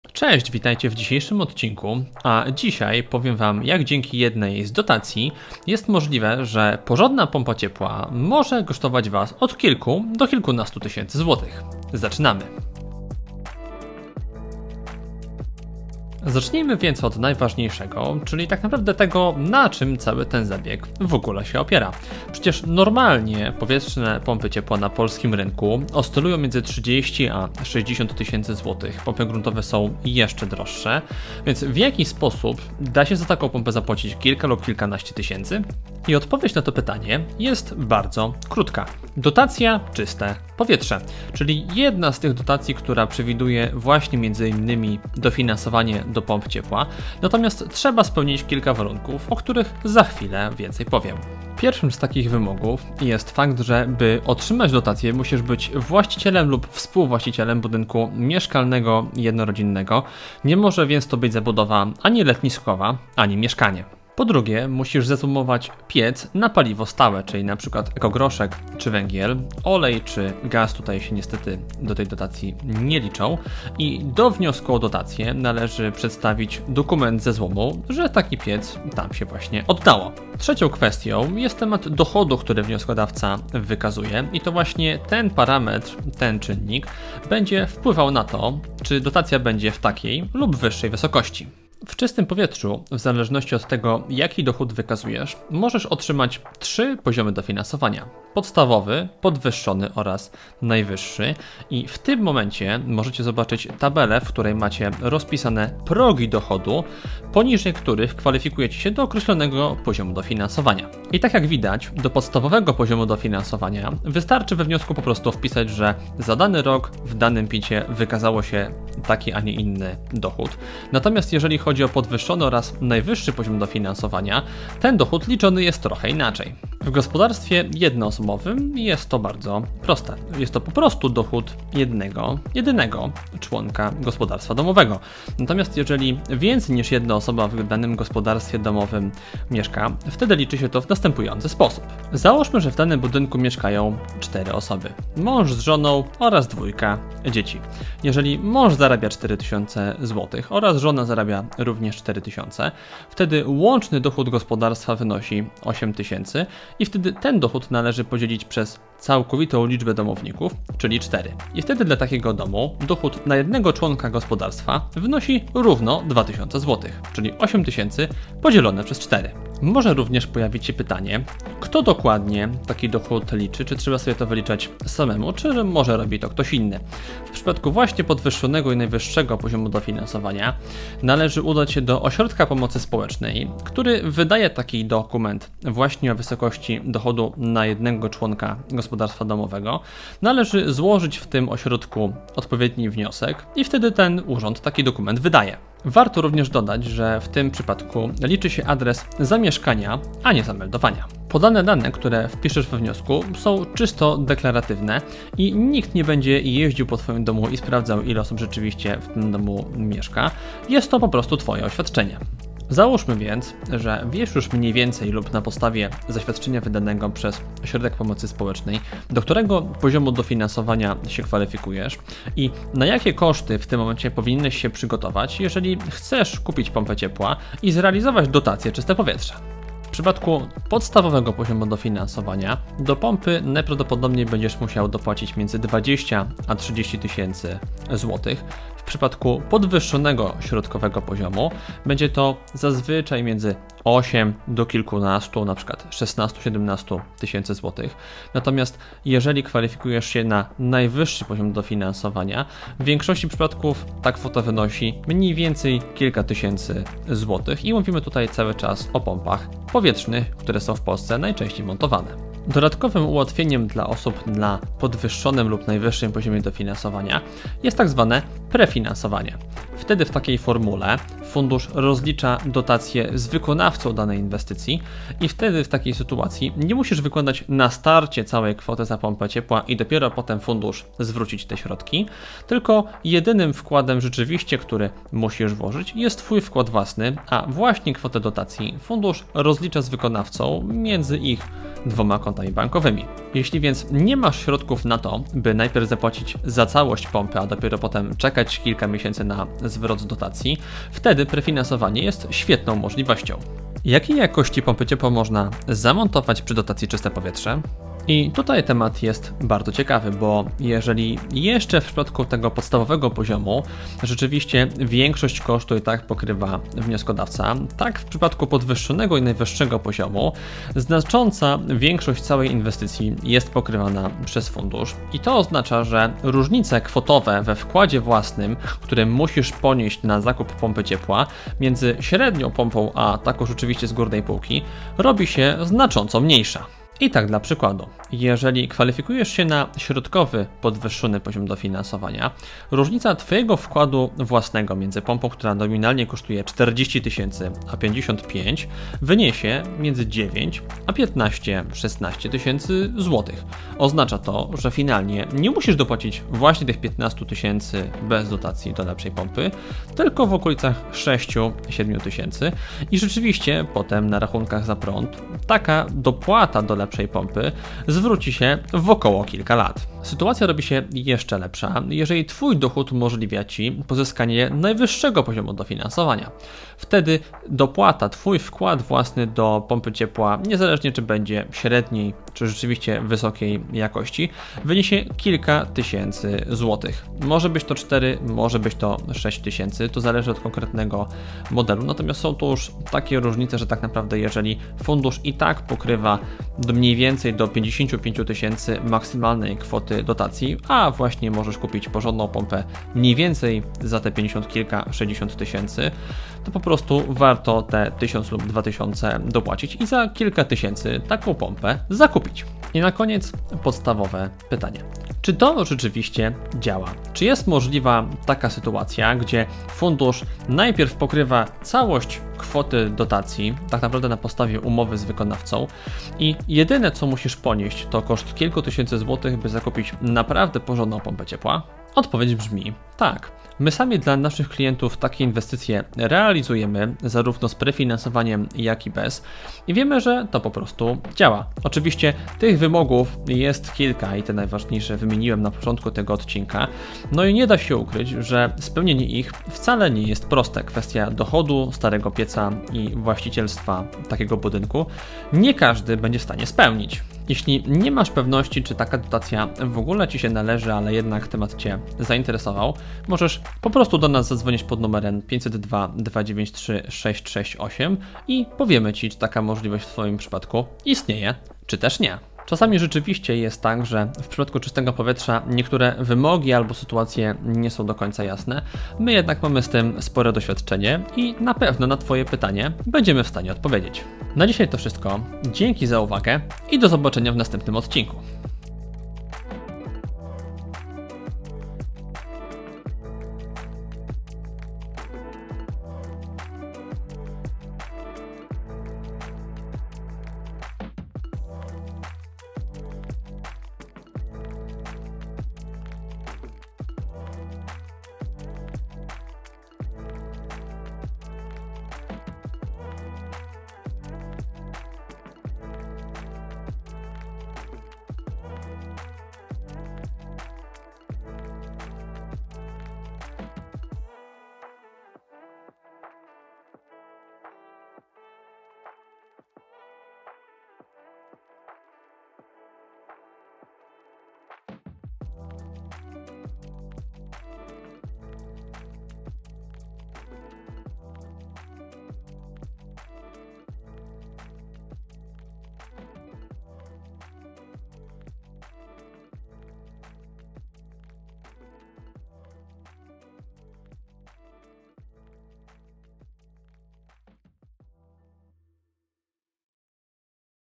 Pompa ciepła PRAWIE za DARMO? To możliwe, jeśli...
Z dzisiejszego odcinka dowiesz się o tym, jak dotacja Czyste Powietrze może obniżyć twój koszt zakupu pompy ciepła.

Wejdź na naszą stronę lub skontaktuj się z nami pod numerem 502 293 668.

Spis treści:
0:00 Wstęp
0:20 Jak można obniżyć koszt zakupu pompy ciepła?
0:52 Warunki dotacji
3:33 Wkład własny do pompy ciepła w Czyste Powietrze
5:04 Jaka pompa ciepła w Czyste Powietrze
6:55 Czy to rzeczywiście działa?